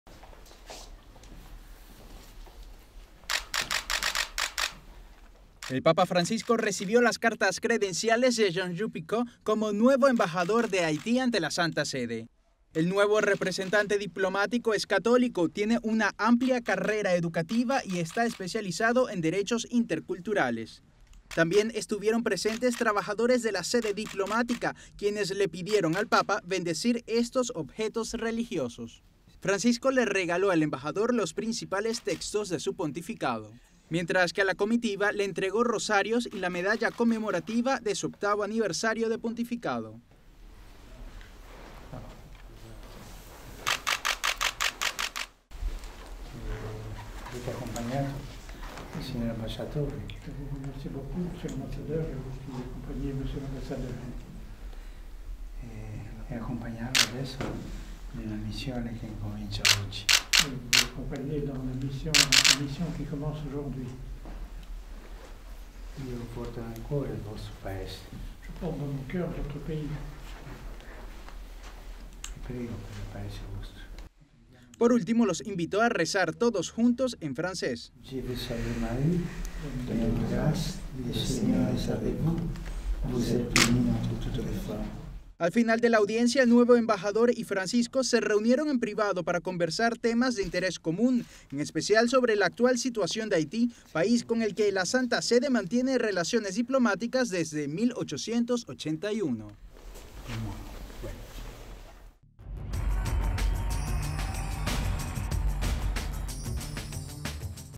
Papa Francisco: Llevo Haití en mi corazón
El Papa Francisco recibió las cartas credenciales de Jean Jude Piquant como nuevo embajador de Haití ante la Santa Sede.